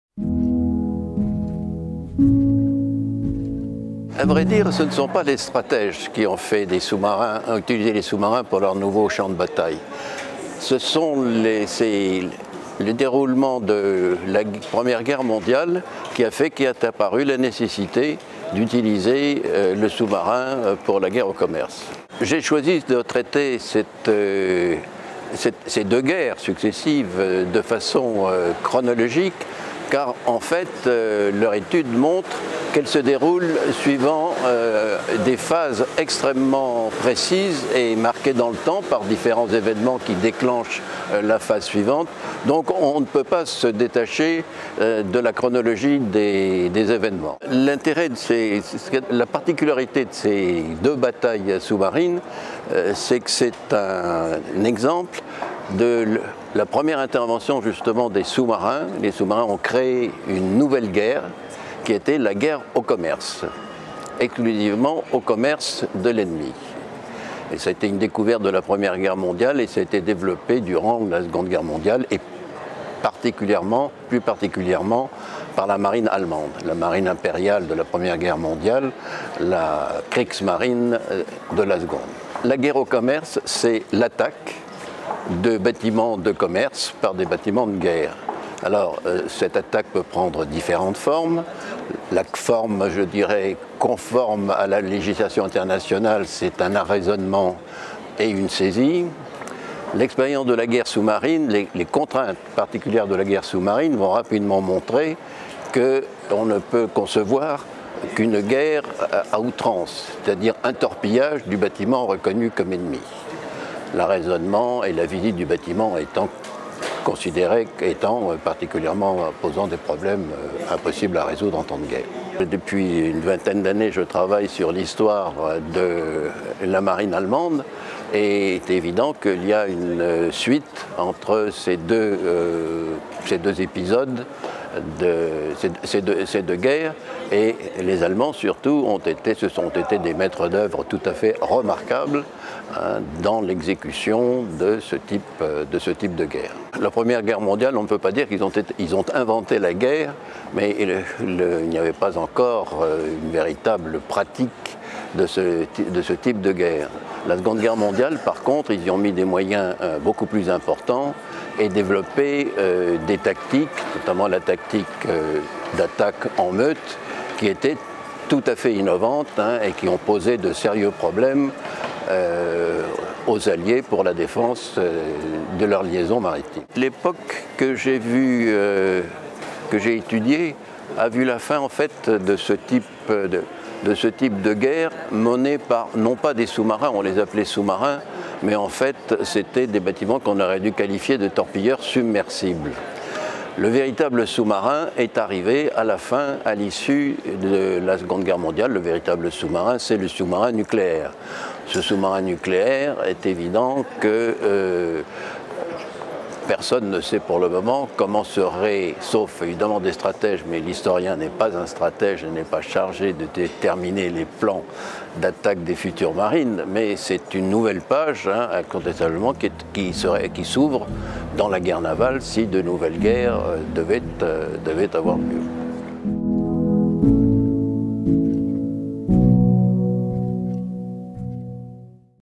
François-Emmanuel Brezet - La guerre sous-marine allemande (1914-1945)
François-Emmanuel Brezet vous présente son nouvel ouvrage "La guerre sous-marine allemande (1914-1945)" aux éditions Perrin.